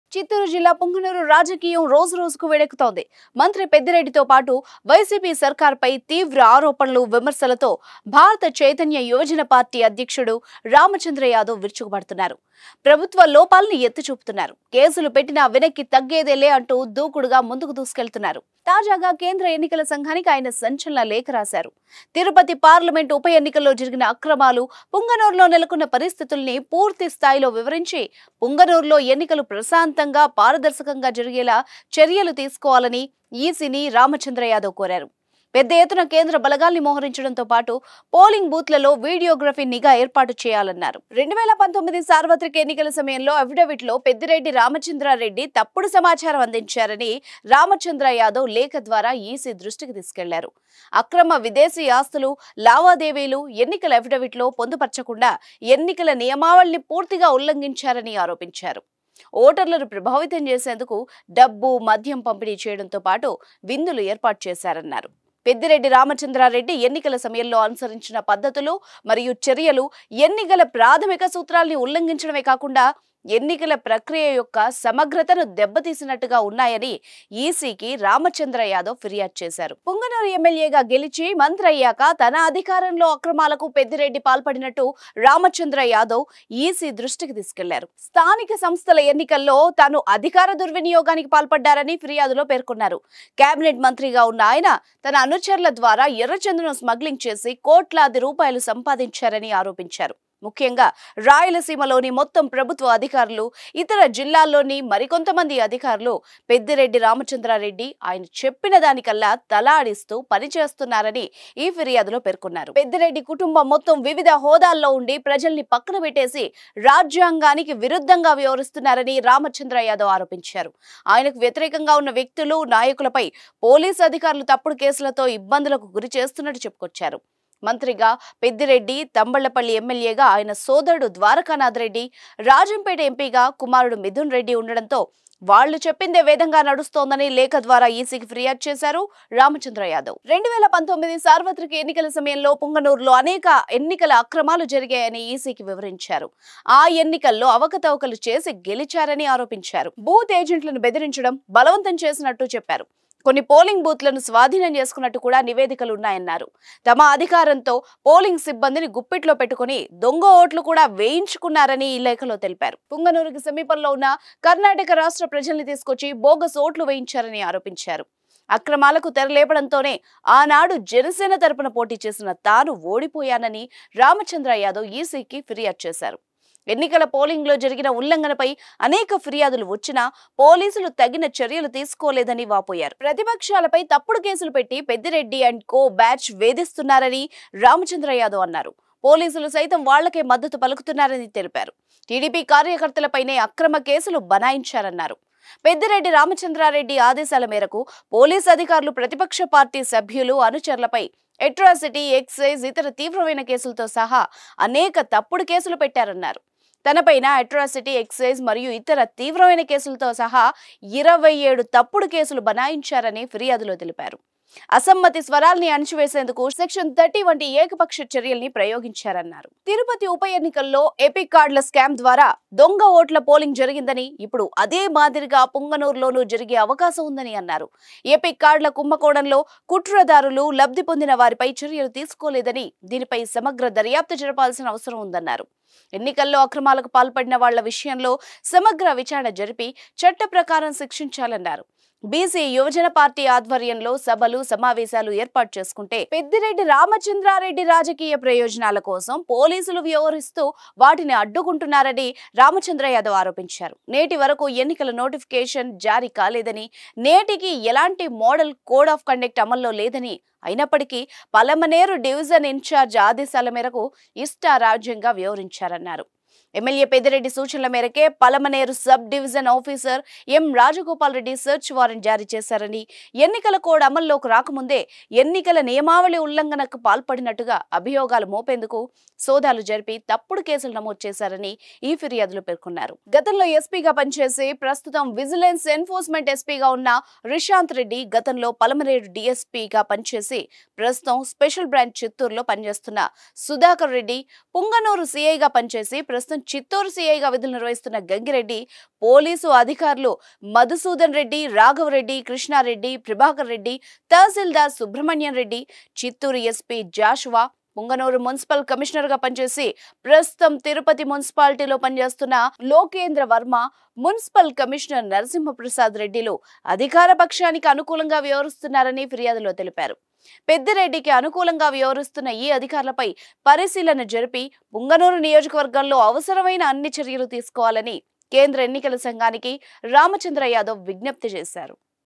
పెద్దిరెడ్డి పై ఫిర్యాదు | BCYP Ramachandra Yadav Complains to EC About Peddireddy
Watch► పెద్దిరెడ్డి పై ఫిర్యాదు..! |  BCYP Ramachandra Yadav Complains to EC About Peddireddy Ramachandra Reddy | @sumantvbheemili